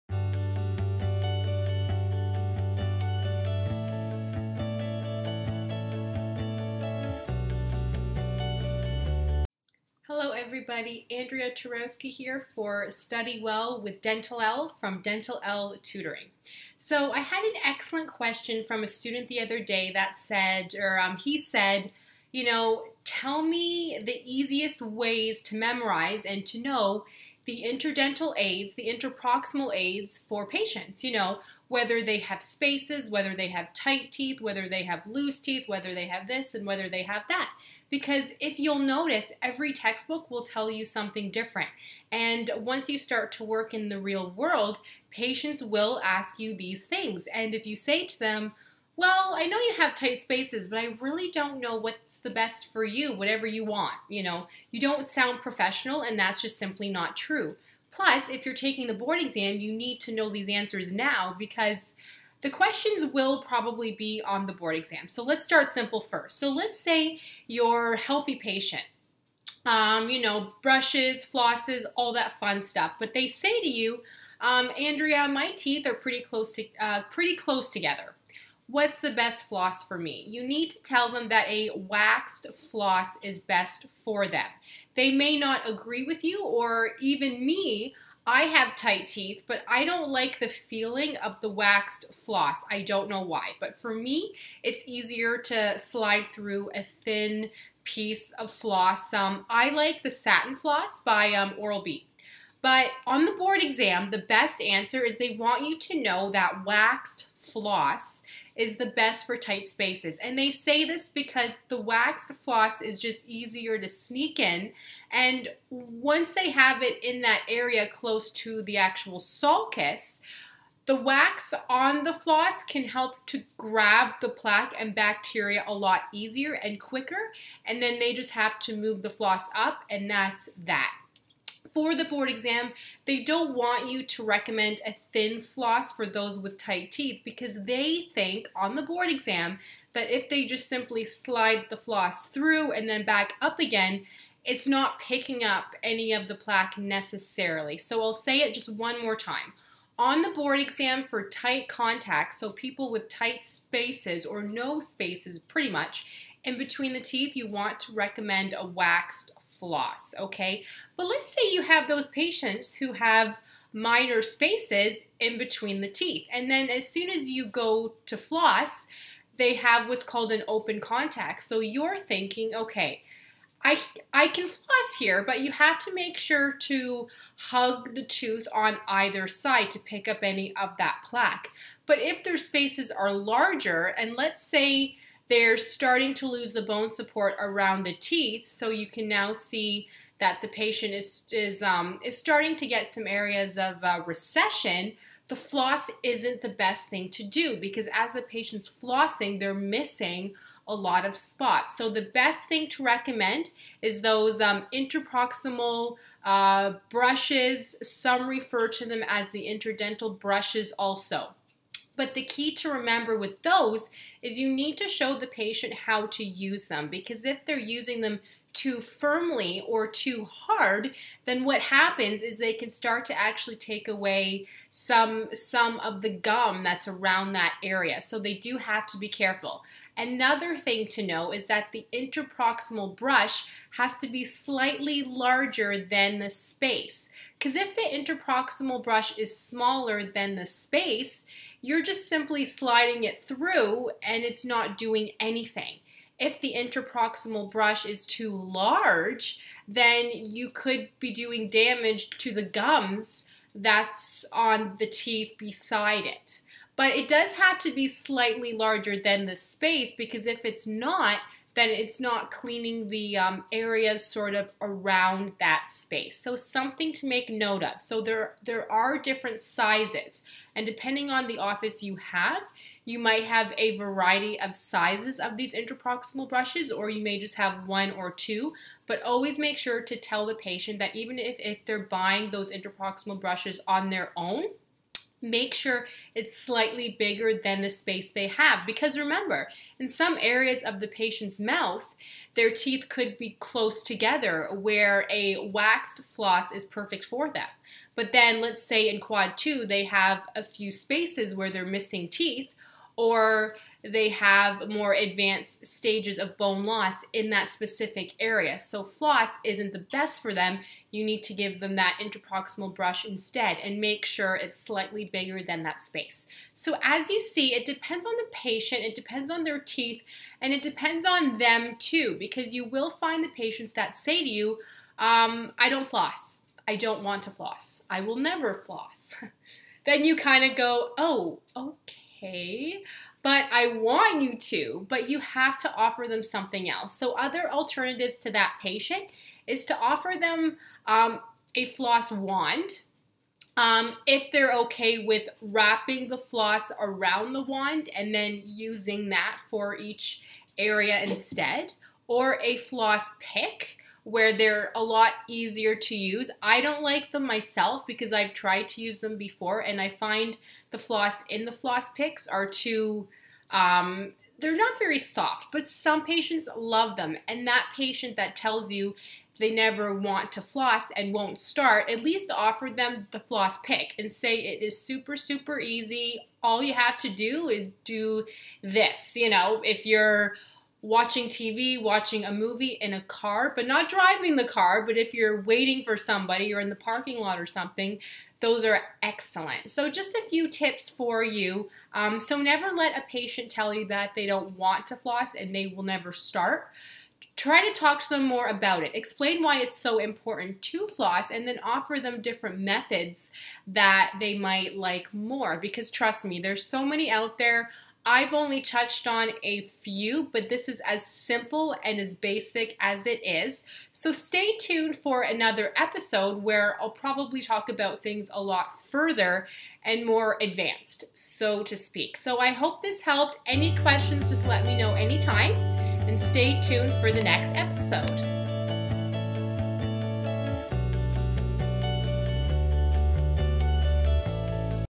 Study Well with Dentalelle - Basics of Interdental Aids
Weekly Videos with Andrea Twarowski from Dentalelle Tutoring - online tutoring for dental hygiene and dental assisting students.  Basics of Interdental Aids in this video - happy studying!